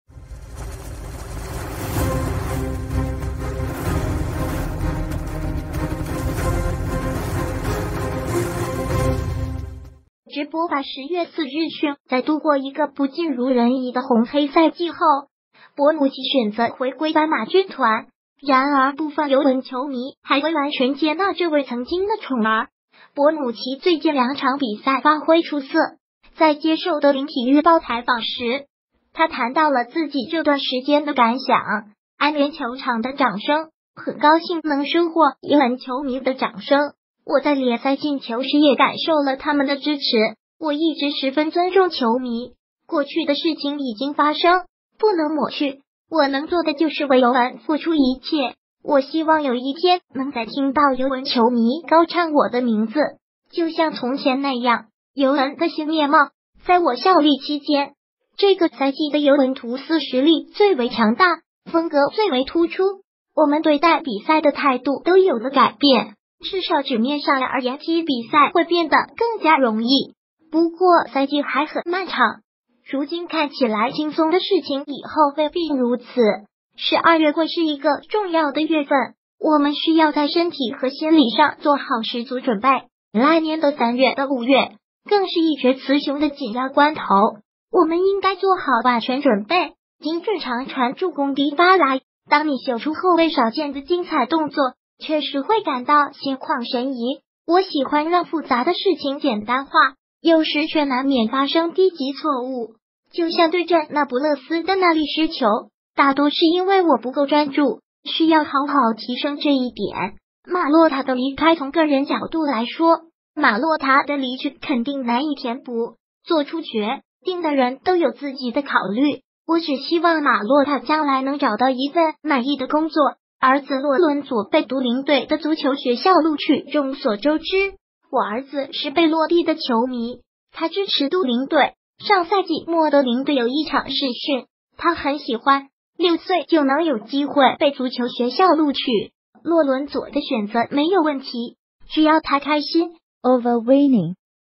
博努奇：效力期间本季尤文最强大 马洛塔的离去难填补
直播吧10月4日讯 在度过一个不尽如人意的红黑赛季后，博努奇选择回归斑马军团，然而部分尤文球迷还未完全接纳这位曾经的宠儿。博努奇最近两场比赛发挥出色，在接受《都灵体育报》采访时，他谈到了自己这段时间的感想。——安联球场的掌声“很高兴能收获尤文球迷的掌声，我在联赛进球时也感受了他们的支持。我一直十分尊重球迷，过去的事情已经发生，不能抹去，我能做的就是为尤文付出一切。”“我希望有一天能再听到尤文球迷高唱我的名字，就像从前那样。”——尤文的新面貌“在我效力期间，这个赛季的尤文图斯实力最为强大，风格最为突出。我们对待比赛的态度都有了改变，至少纸面上而言踢比赛会变得更加容易。”“不过赛季还很漫长，如今看起来轻松的事情以后未必如此。12月会是一个重要的月份，我们需要在身体和心理上做好十足准备。来年的三月到五月，更是一决雌雄的紧要关头，我们应该做好万全准备。”——精准长传助攻迪巴拉“当你秀出后卫少见的精彩动作，确实会感到心旷神怡。”“我喜欢让复杂的事情简单化，有时却难免发生低级错误，就像对阵那不勒斯的那粒失球。大多是因为我不够专注，需要好好提升这一点。”——马洛塔的离开“从个人角度来说，马洛塔的离去